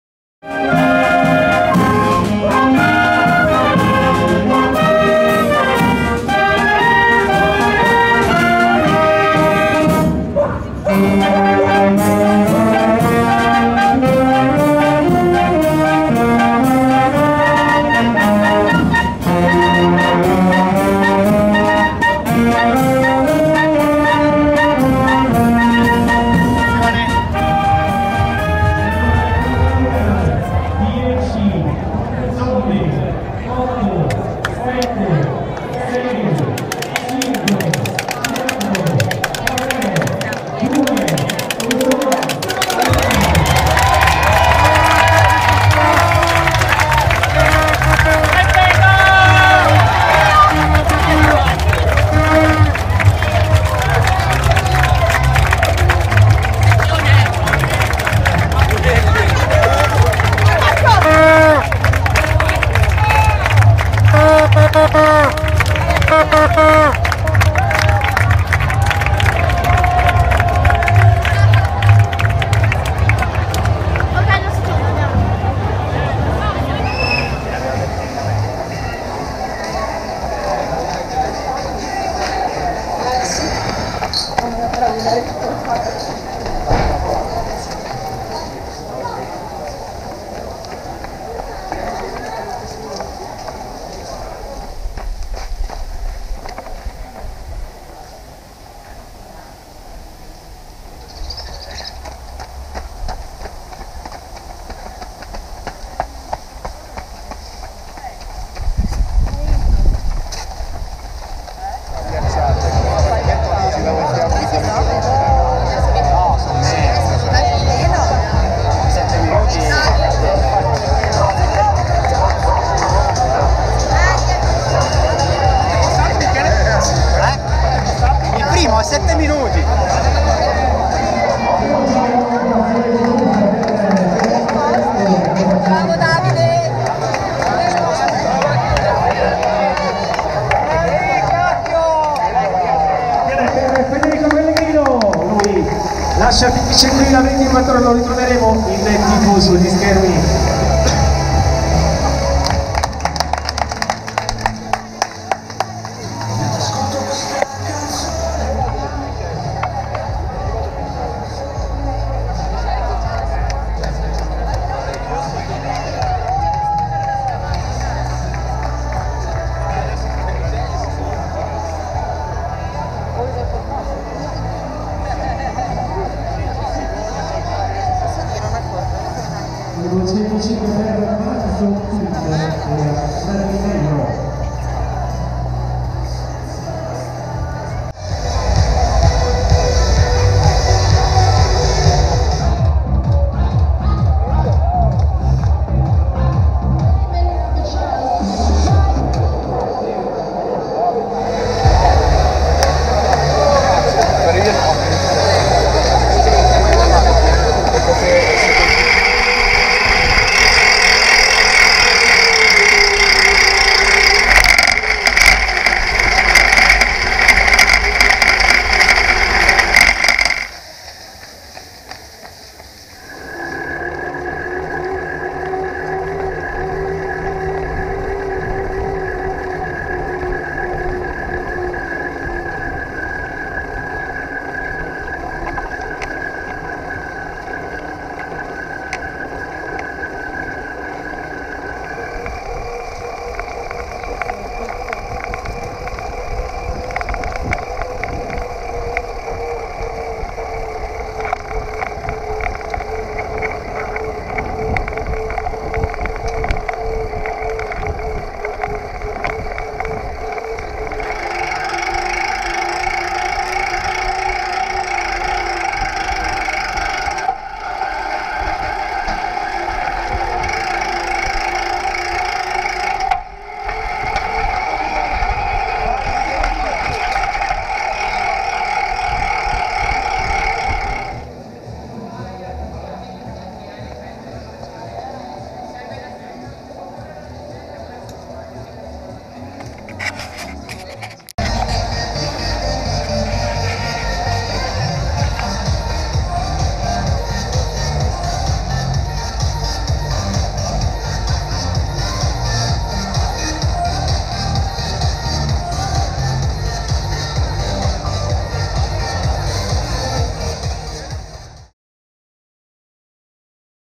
Edileco RUN24 corsa a staffetta di 24 ore nella Città di Aosta
Edileco RUN24 corsa a staffetta di 24 ore nel cuore di Aosta lungo un percorso di 3 km che attraversa il centro storico.